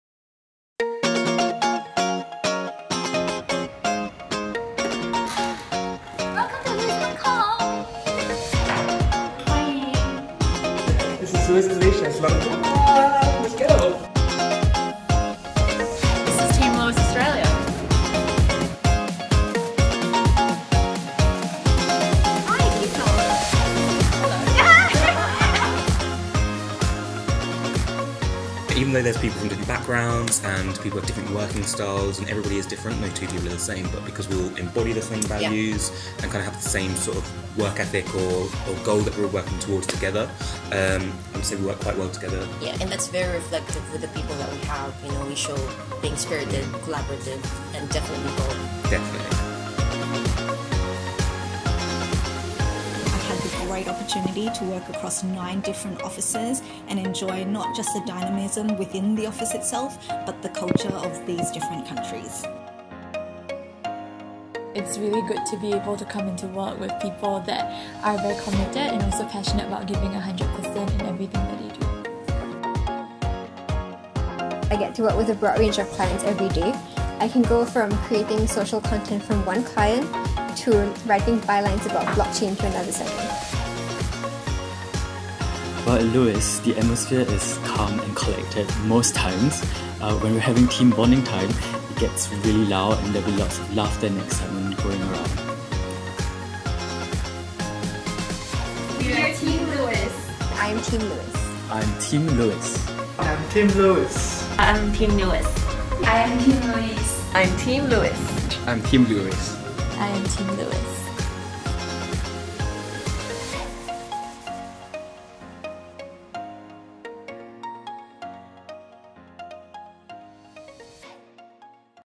Meet Team LEWIS APAC
Step inside our LEWIS Asia-Pacific offices in Beijing, Hong Kong, Kuala Lumpur, Shanghai, Singapore and Sydney and find out what Life @ LEWIS is like.